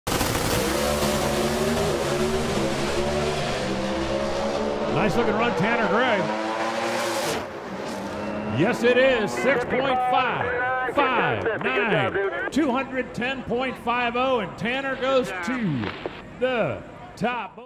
Tanner Gray goes to the top in Charlotte
Tanner Gray currently holds the top spot after driving to a 6.559 at 210.50 during his first Pro Stock pass of the day. He is seeking his fourth No. 1 qualifier of the season.